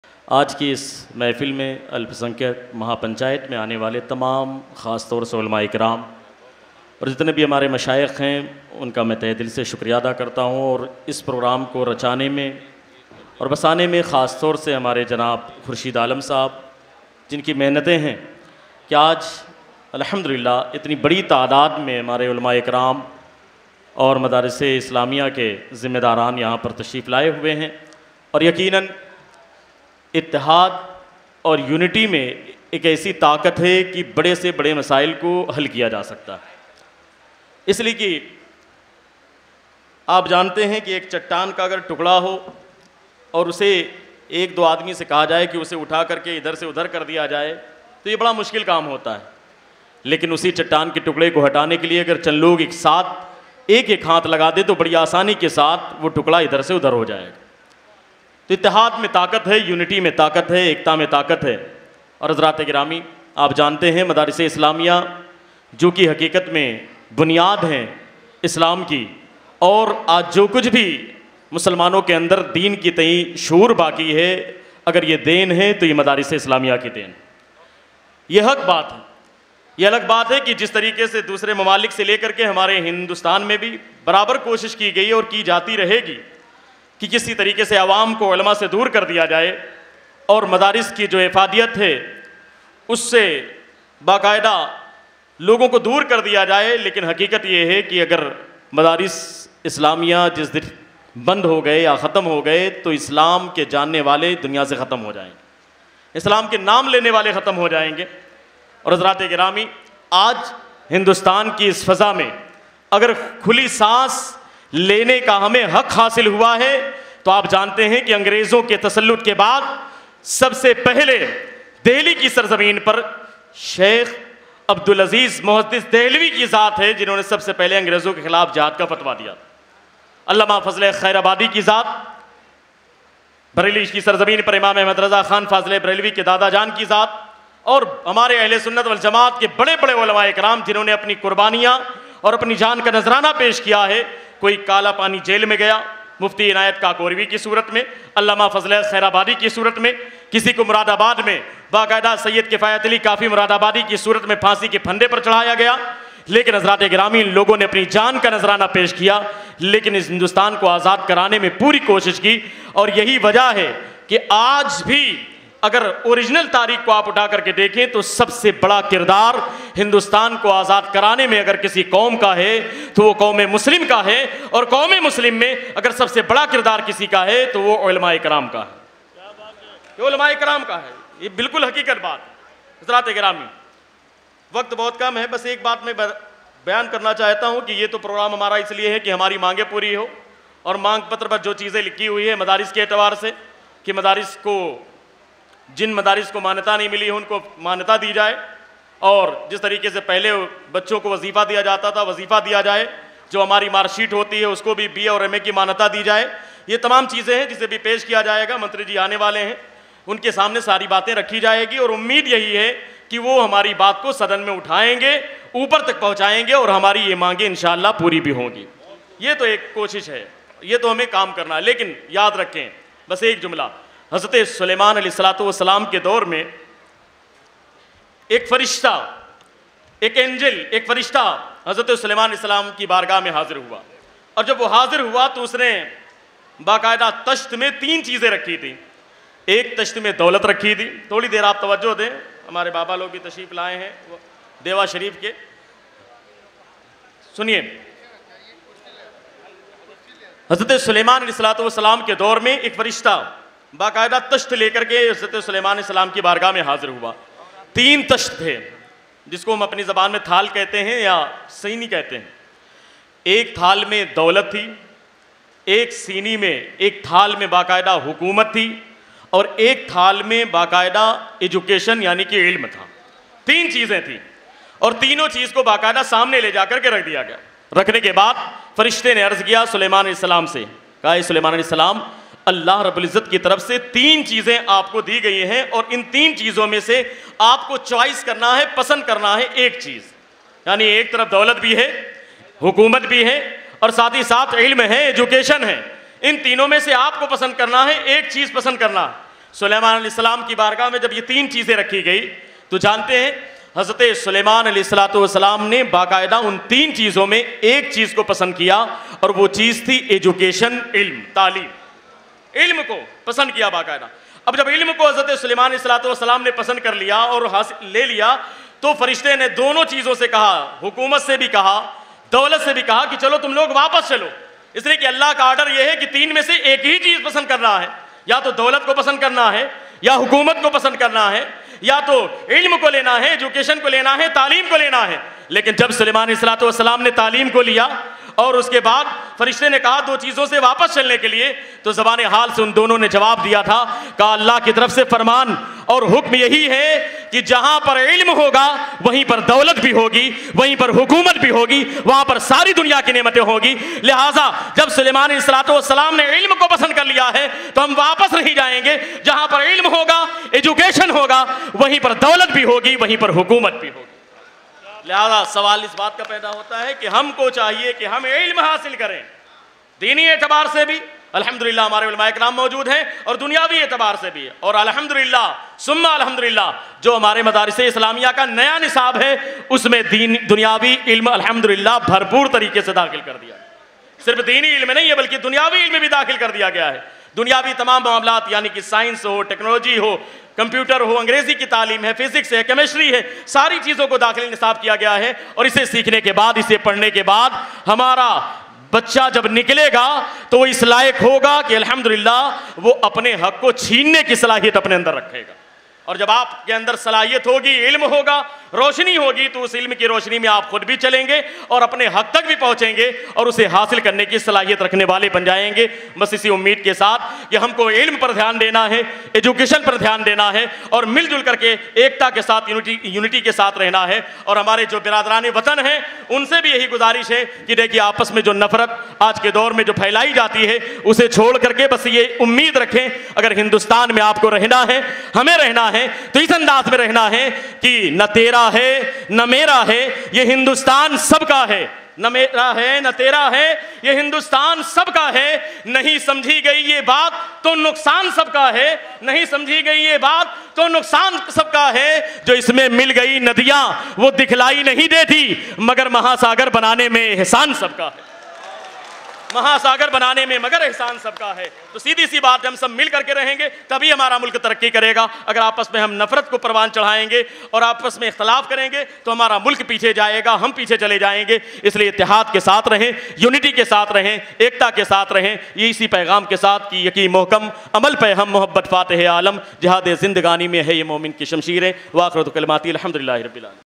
मुफ़्ती साहब ने मदरसों को लेकर की बड़ी मांग | Mufti Shahnawaz Azhari
आप अपनी न्यूज़ हम तक पहुंचा सकते हैं.